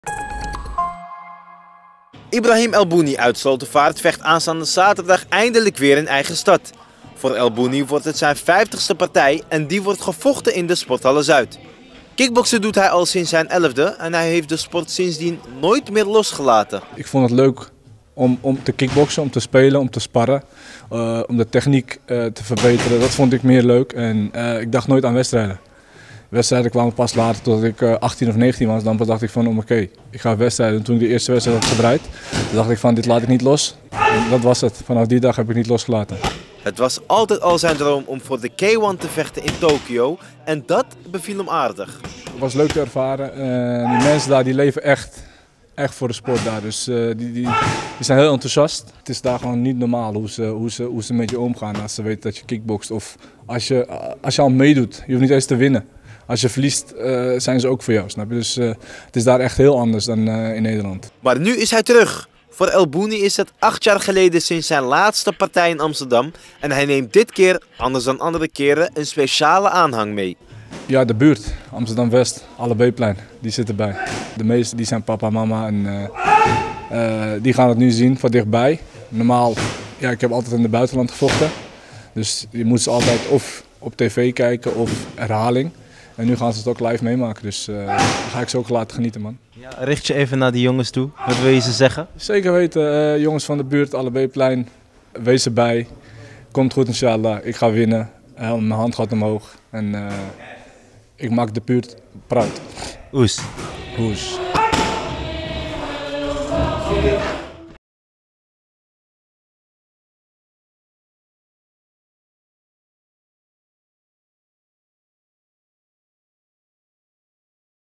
Glory-vechter Ibrahim el Bouni na 8 jaar weer in Amsterdam
Kickbokser Ibrahim El Bouni vecht na acht jaar weer in zijn thuisstad Amsterdam. De laatste keer dat hij in Amsterdam de ring betrad was in 2014. Na de hele wereld rondgereisd te hebben om te vechten, is hij aanstaande zaterdag 5 november weer terug. Ibrahim "Mister Cool" El Bouni vecht de hoofdpartij van Glory Rivals 3, georganiseerd in de Sporthallen Zuid.

El Bouni is op 13 jarige leeftijd begonnen met het kickboksen en had niet gelijk het doel om wedstrijden te vechten. Zijn doel was meer om, zoals hij het zelf zegt "een beetje te spelen, een beetje te sparren". Rond zijn negentiende vocht hij zijn eerste partij en dat smaakte naar meer. Vanaf dat moment wist hij het zeker, vechten van meer wedstrijden, voor geld. "Het was altijd al mijn droom om voor de K-1 te vechten."

En dat lukte aardig. Meer dan de helft van zijn partijen heeft hij gevochten in het buitenland waaronder in Azië, bij de K-1 in Tokyo en One Championschip die organiseert over heel Azië. Met 49 partijen waarvan 39 afgesloten met een winst staat hij aanstaande zaterdag op 5 november weer de ring in tegenover Mohamed Balli. Als El Bouni wint wordt het zijn 50e winst in zijn carrière.

Omdat zijn partijen niet vaak in Nederland zijn, hebben 'de jongens uit de buurt', Slotervaart, deze keer geen excuus om er niet bij te zijn. "De meeste zijn al papa, die gaan het nu zien van dichtbij. Ik heb vaker in het buitenland gevochten en daarom moesten zij de wedstrijden altijd terugkijken. Ik ga ze laten genieten 5 november", zegt El Bouni en hij spreekt speciaal de jongens uit de buurt van het August Allebéplein aan.

El Bouni staat momenteel op plaats 3 van de licht-zwaargewichts-ranglijst van Glory en dat betekent dat hij in de buurt komt voor een titelgevecht. Recent heeft El Bouni partijen gewonnen van Badr Ferdaous en Clyde Brunswijk op eerdere Glory gala's. Over wat er na deze partij komt wil El Bouni nog niet aan denken. Hij wil zich eerst focussen op deze tegenstander.